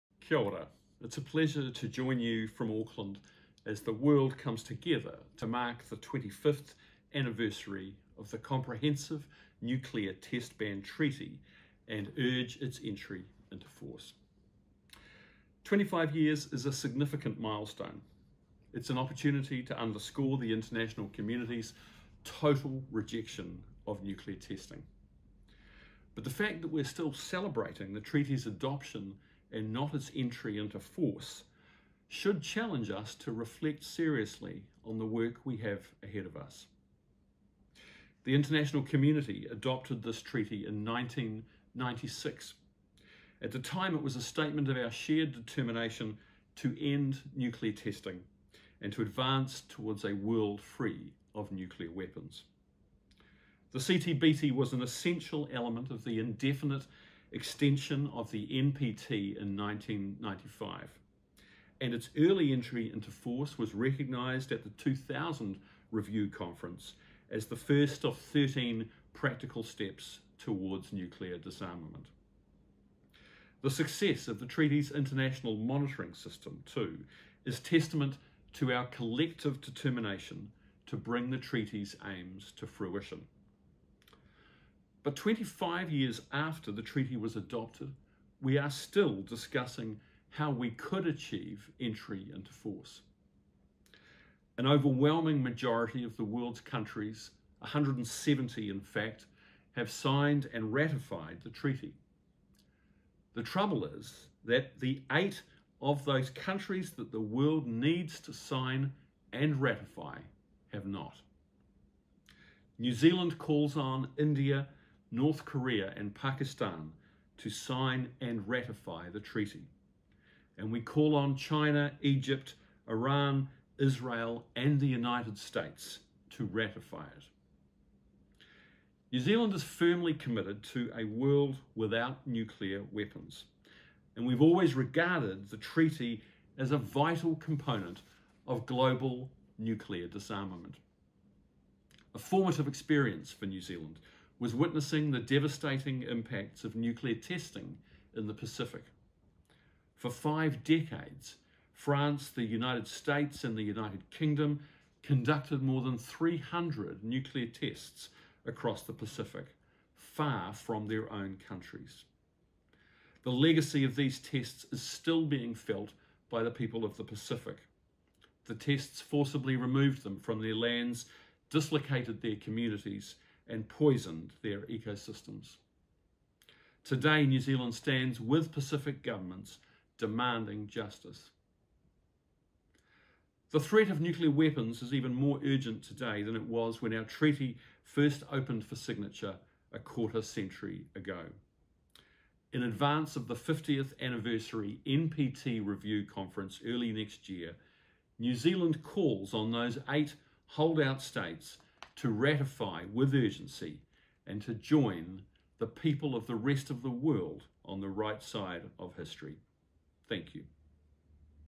Phil Twyford - CTBTO Conference video statement
Video statement for the Comprehensive Test Ban Treaty Article XIV (CTBTO) conference by New Zealand Minister for Disarmament and Arms Control Phil Twyford.